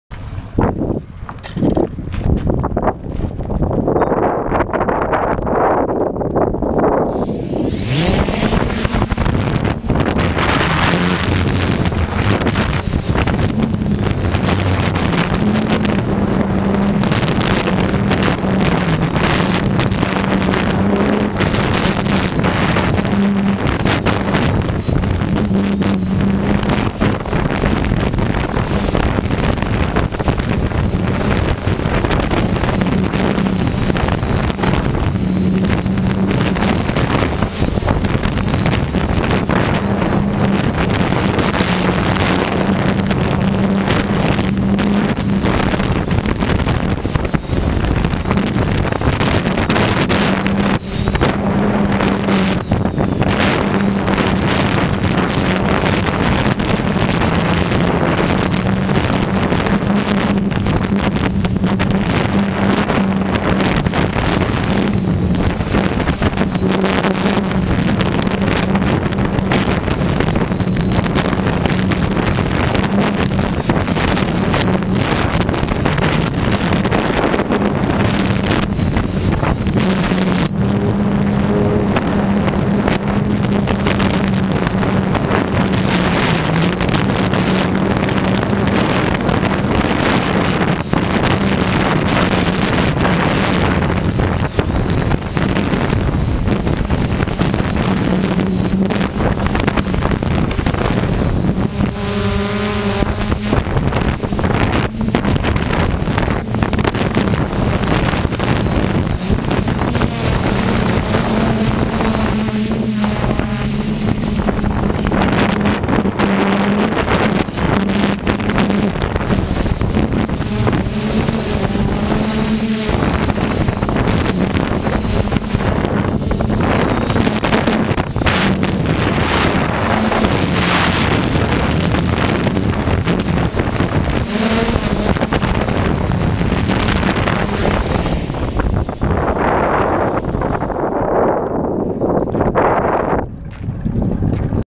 Mini B (Flathead V8 powered) Aerial Video
This a custom built 1949 Ford Flathead V8 powered 1983 Case 226 garden tractor with Allis-Chalmers B sheet metal and parts and trailer in tow.